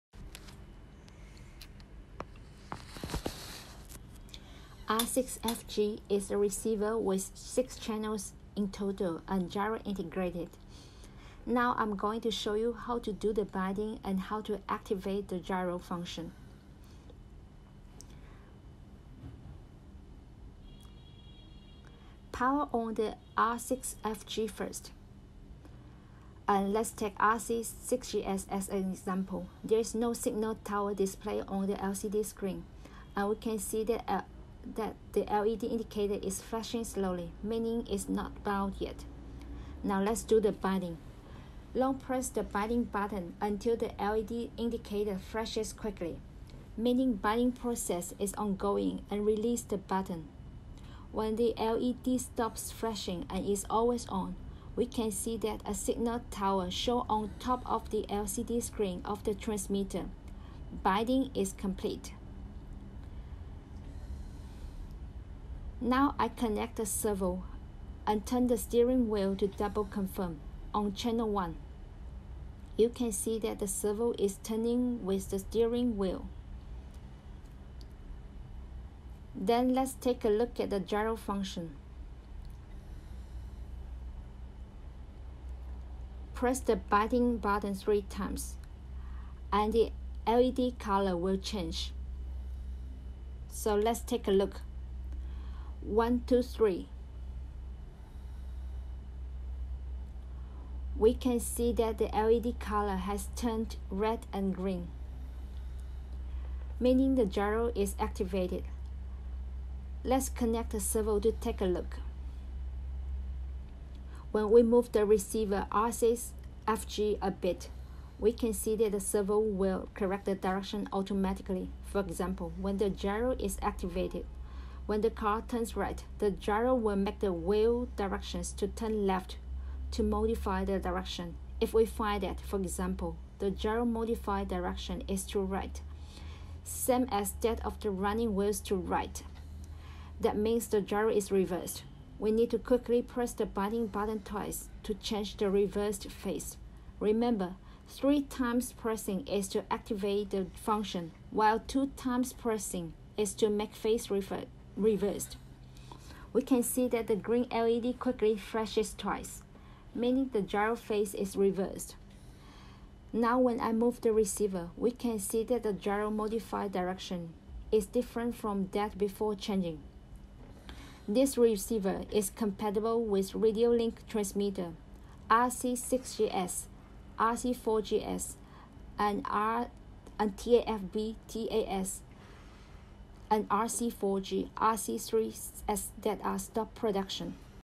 How to| Make RadioLink receiver R6FG bind with transmitters
R6FG is compatible with T8FB/T8S/RC6GS V2/RC4GS V2/RC6GS/RC4GS/ RC4G/RC3S
RadioLink radio control systems are not open sourced so RadioLink transmitters are ONLY compatible with RadioLink receivers.